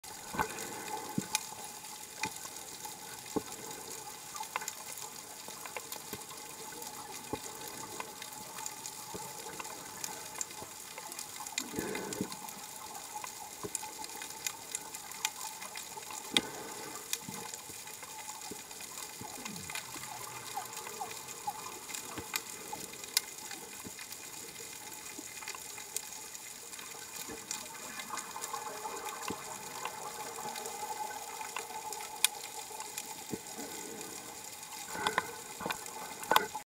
Giant Black Sea Bass Encounter
Diving off Santa Cruz Island, California we stumble across a black sea bass. It demonstrates how typical wildlife encounters come about in Scuba Diving.